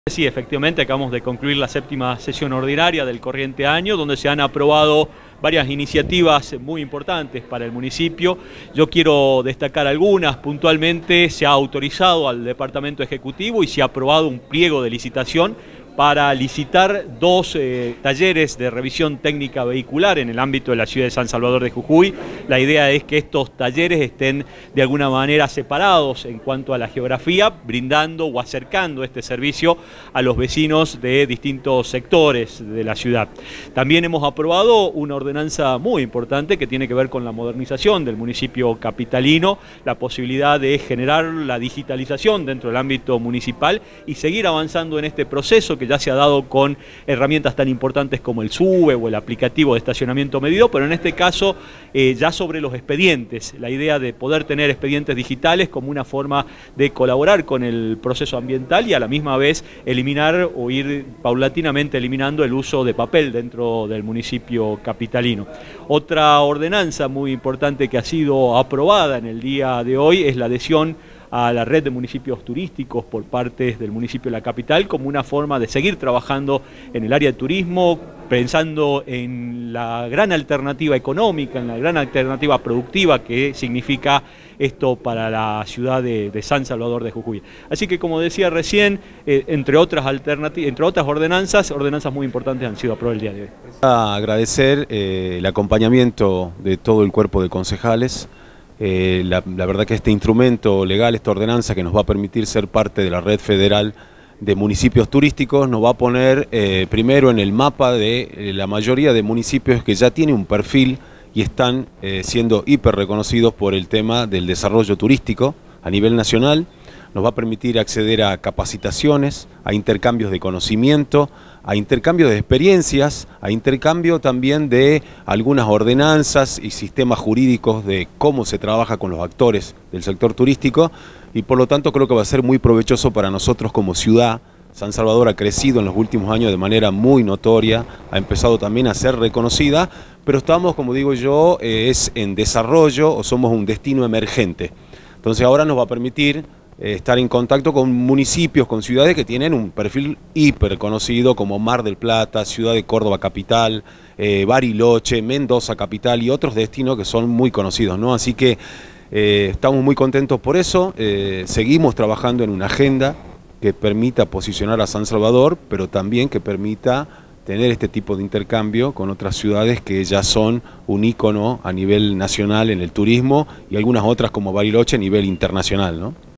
Sesión Ordinaria 24-05-24
Aprobaron la integración a la Red Federal de Municipios Turísticos

En una nueva Sesión Ordinaria del presente año legislativo, el Concejo Deliberante aprobó la Ordenanza Nº 8016, referente a integrar al Municipio de San Salvador de Jujuy a la Asociación Civil Red Federal de Turismo de la República Argentina. Por este motivo, la sesión contó con la presencia de referentes de la Secretaría de Turismo de la comuna local.